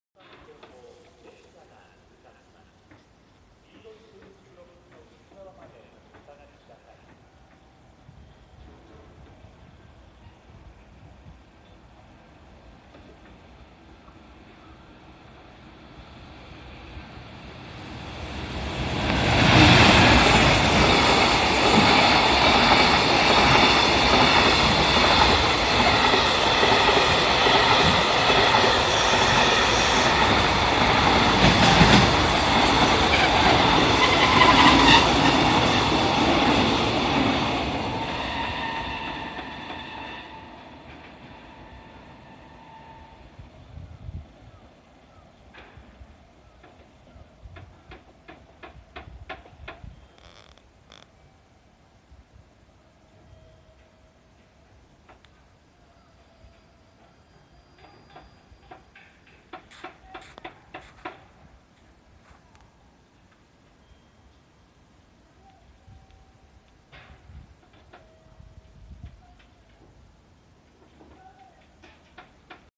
5060ﾚ EF210-149号機牽引カンガルーライナー(上り)相見駅通過 ある意味西濃コンテナのみ積載
EF210電気機関車が牽引しているカンガルーライナーが相見駅を通過してゆくところを撮影してみました

ある意味西濃運輸のコンテナのみでした···